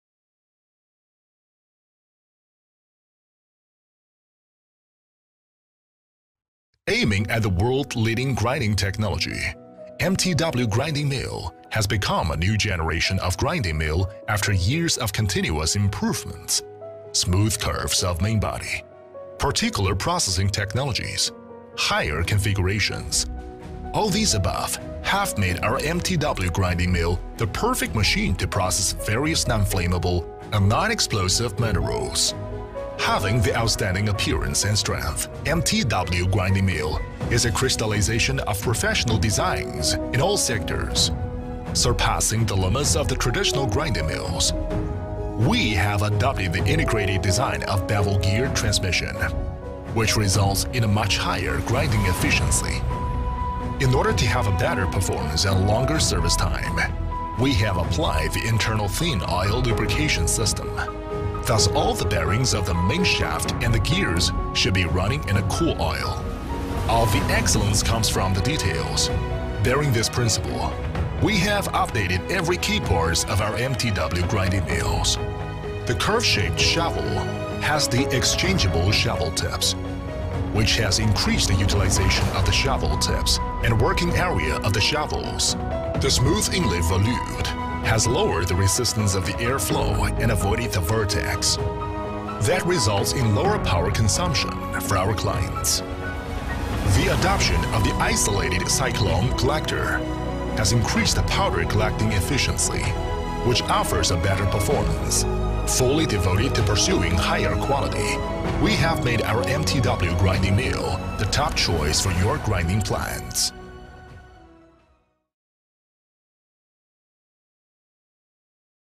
MTW Serie de Molino Trapezoidal
MTW series de molino trapezoidal es el molino del último modo, desarrollado por los expertos de nuestra empresa en base de 10 años del estudio de la máquina de molienda. Posee muchas patentes de propiedad independiente, como el engranaje del pinchazo,el sistema de lubricación de aceite interno,el tubo de aire de forma de arco. Debido a todos estos factores ,el MTW series de molino trapezoidal es el molino principal de la industria de molienda en el mundo.
1. El aspecto exquisito.
2. La transmisión completa de engranajes cónicos.
3. Sistema interno de lubricación de aceite.
4. Tubo curvado.
5. Pala de superficie curvada con la paleta intercambiable.
6. Colector del ciclón aislado.
7. No hay resistencia voluta de entrada (puerta pequeña de observación sin vórtice).